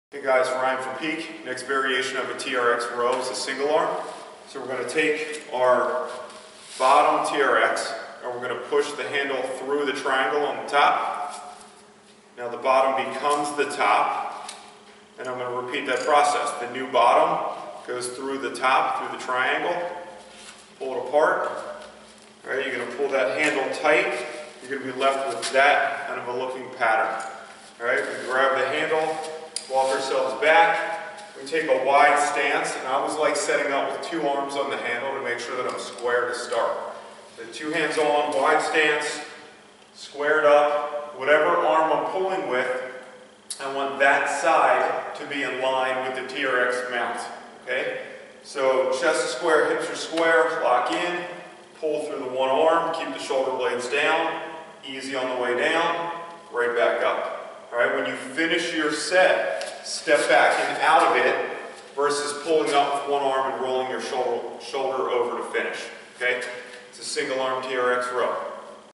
TRX Row - Single Arm
Challenging horizontal pull (row) with torsion control component.  Keep hips and shoulders square as you are pulling with lat, bicep, and retracting shoulder blade.  Great ex. for rotational sport athletes and functional strength training.